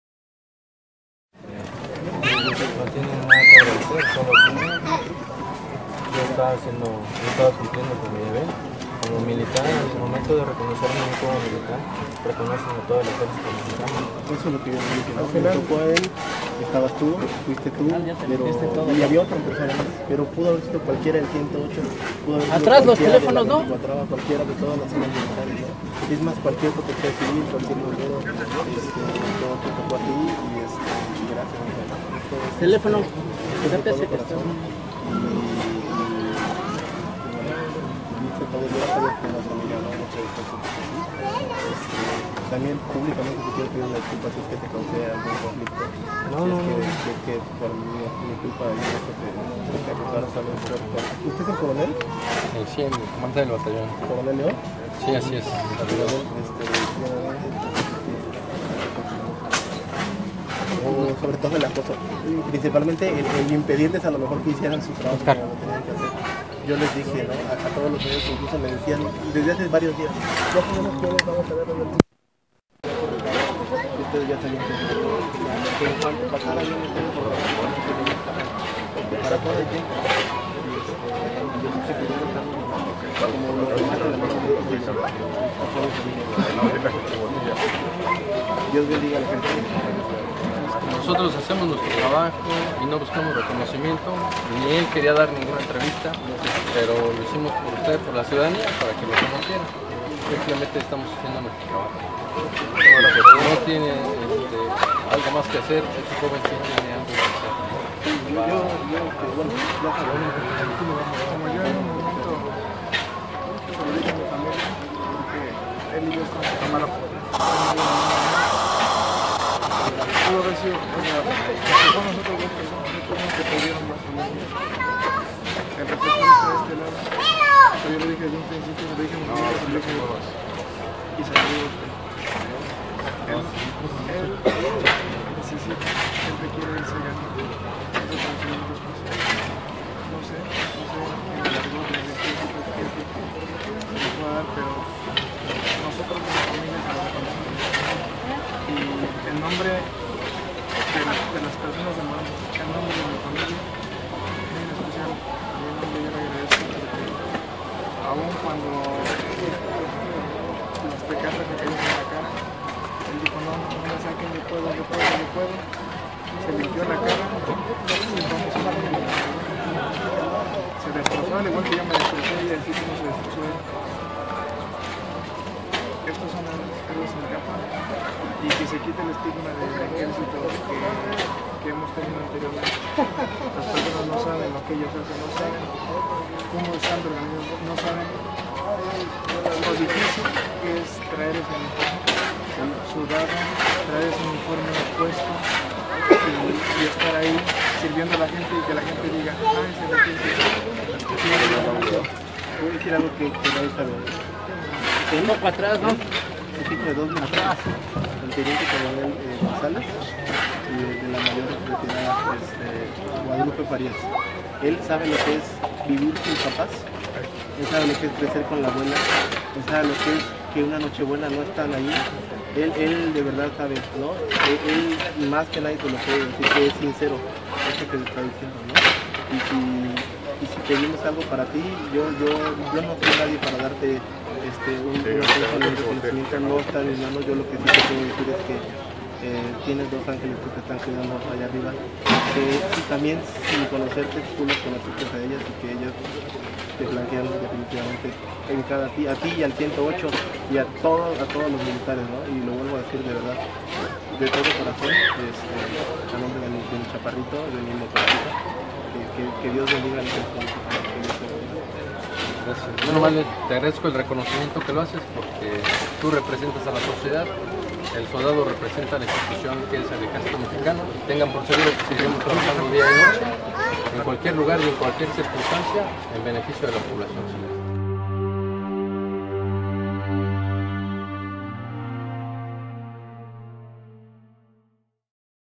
Fotógrafo se reúne con el soldado que trató de salvar a su familia
Fecha: 26/09/2017
Hora: 15:32
Lugar: Jojutla, Morelos
Tema: Sismo 7.1 grados

Llanto y esfuerzo de soldado fue reconocido con emotivo abrazo
Marco Gil Vela agradeció al militar Martín Moctezuma Luis Hernández por rescatar los cuerpos de su esposa e hija de entre los escombros en Jojutla; 'cumplía con mi deber', dijo el militar a los medios de comunicación

26/09/2017 20:14 PEDRO TONANTZIN / EFE
COMPARTIR
SÍGUENOS

1 / 5
Foto: @24_morelos
1 de 5
JOJUTLA, MORELOS

A siete días del terremoto que sacudió Jojutla y que dejó más de 74 personas sin vida en Morelos, Marco Gil Vega se pudo reunir con el soldado anónimo que llegó en los primeros minutos de la tragedia a rescatar a su esposa e hija.

El hombre de uniforme dejó de ser anónimo y dijo que él también ha sufrido en carne propia los efectos de los sismos registrados este mes de septiembre.
Video de: @UnoTVNoticias